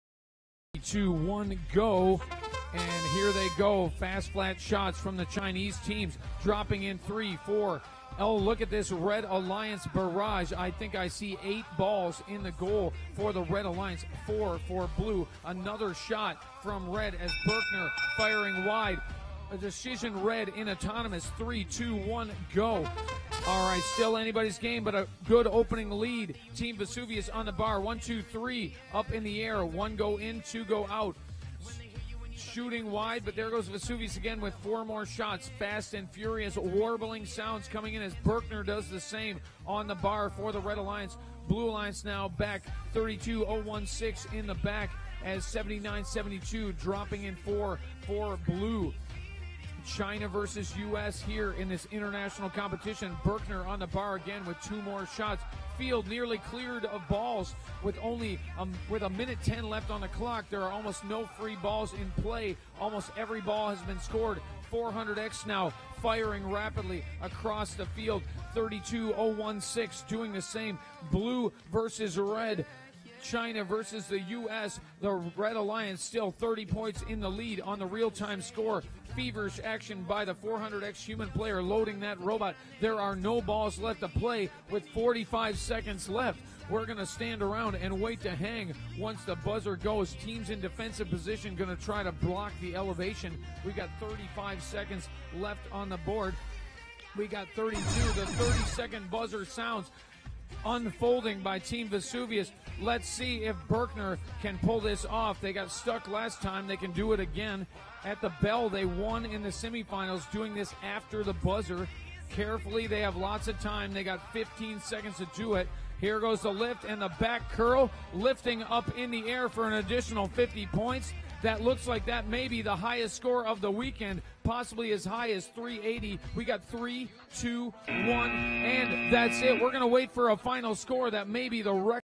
VEX Worlds 2016 - VRC HS Engineering - Finals 1 (1366 400X 2105C) 333 vs 217 (32016A 7972D 2Z)
VEX Worlds 2016 - VRC HS Engineering - Finals 1

Red - Score 333:
1366 - Berkner Fearless Robotics (Berkner STEM Academy, Richardson, Texas, United States)

400X - Floyd IV - Vesuvius (Galveston I.S.D. Robotics, Galveston, Texas, United States)

2105C - Swampbotics (Ware County High School, Waycross, Georgia, United States)

Blue - Score 217:
32016A - North China Team (North China Team, Xi'an, N/A, China)

7972D - Gaoxin NO.1 HSch D (Xi'an Gaoxin NO.1 High School, xi'an, China)

2Z - Robosavages Z (Gladstone Secondary School, Vancouver, British Columbia, Canada)